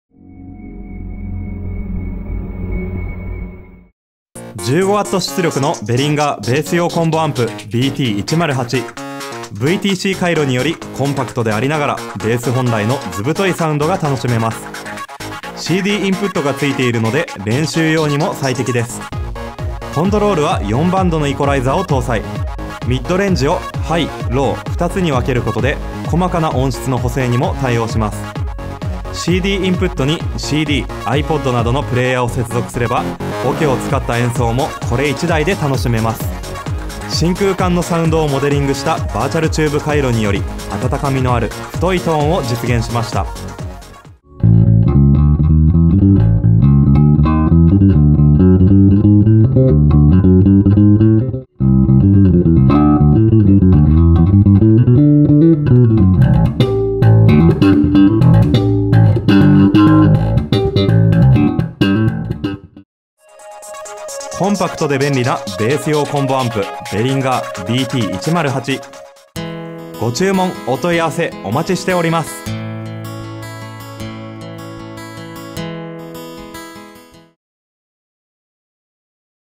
BEHRINGER / ベースアンプ コンボタイプ BT108
ベース用コンボアンプBT108のご紹介です。15W出力のコンパクトサイズでありながら、VTC回路によるベース本来の図太いサウンドが楽しめます。もちろん練習に欠かせないCDインプットやヘッドホン端子も装備。オケに合わせたパフォーマンスにも便利です。気になるサウンドはこちらのムービーをご覧ください！